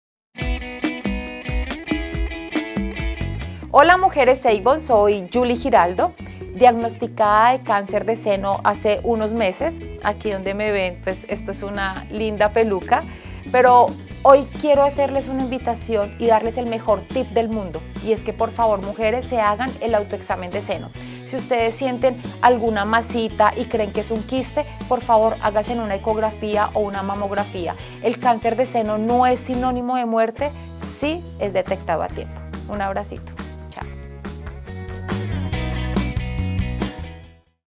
Tip #6: hazte el autoexamen.
Yuli Giraldo, asesora de imagen y personal shopper nos comparte sus tips de estilo.